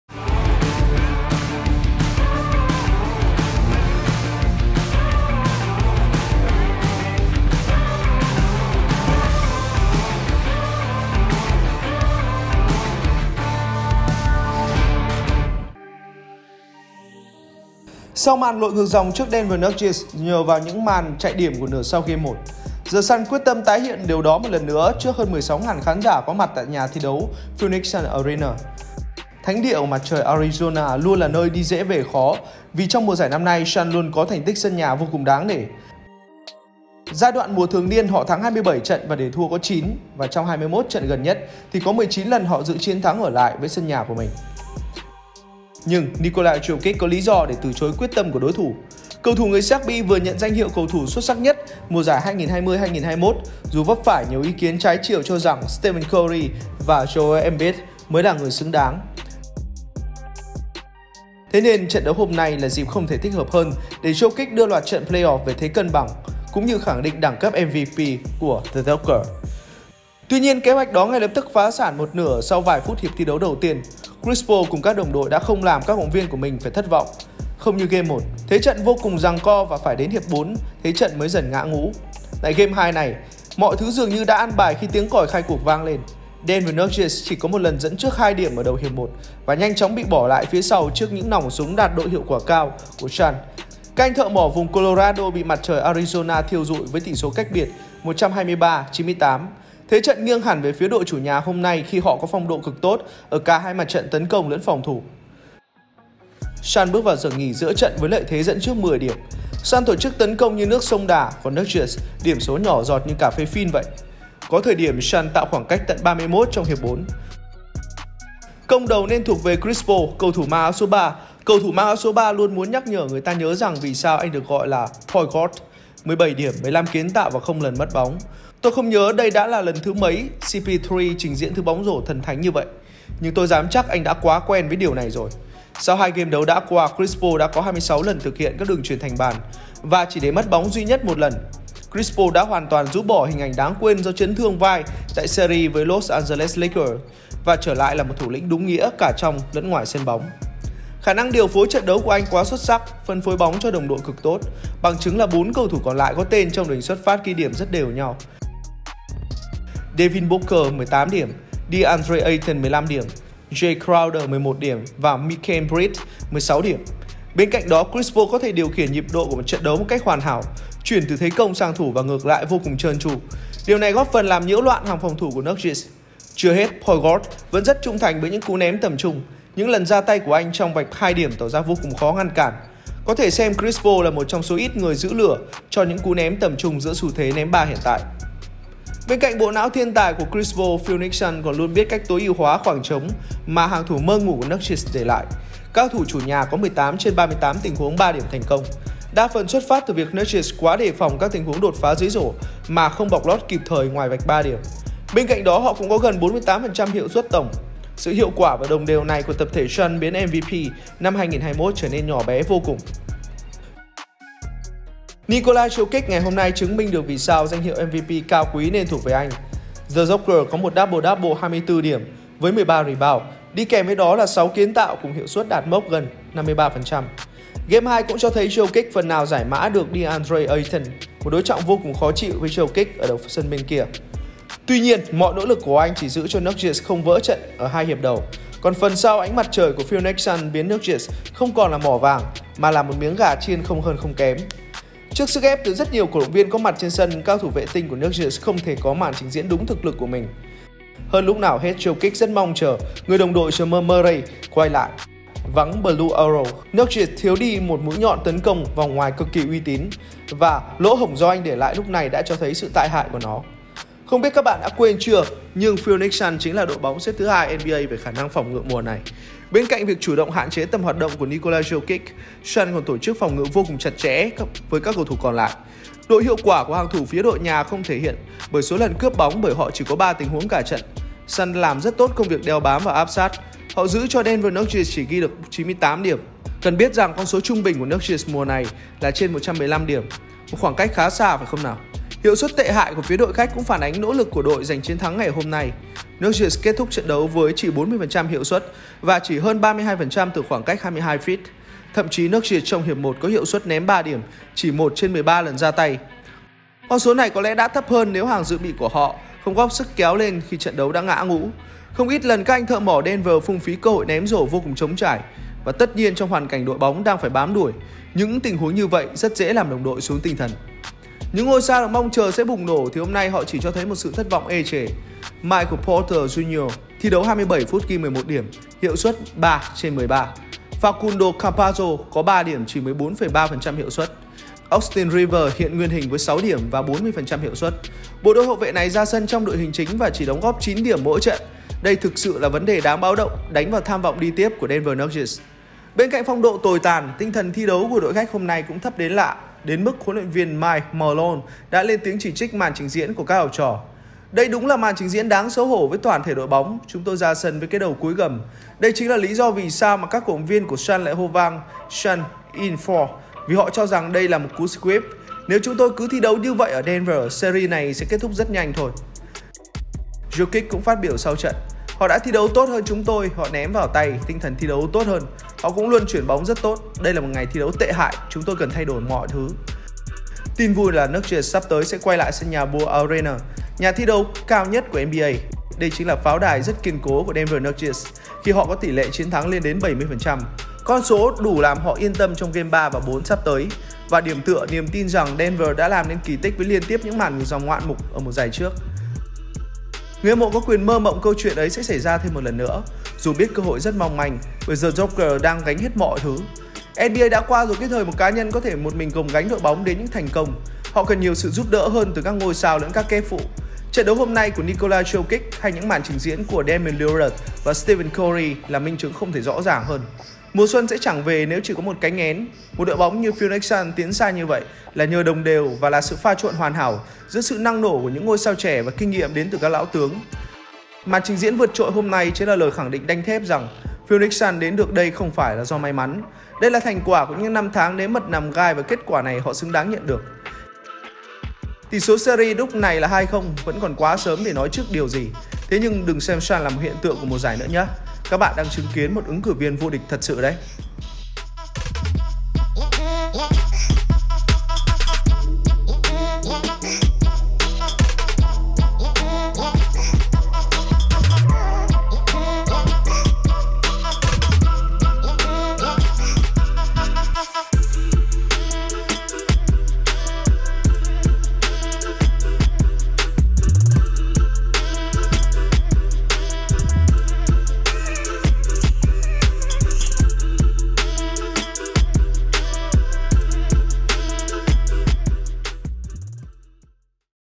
NBA PLAYOFFS: PHOENIX SUNS HẠ GỤC DENVER NUGGETS TRONG NGÀY MVP JOKIC QUÁ ĐƠN ĐỘC
Nắm giữ lợi thế sân nhà, cùng phong độ đang bay cao sau chiến thắng game 1. Phoenix Suns không quá vất vả để hạ bệ các vị khách đến từ Denver với khoảng cách lớn. Point God Chris Paul đã cho thấy The Joker còn non như thế nào bằng màn trình diễn quá đẳng cấp.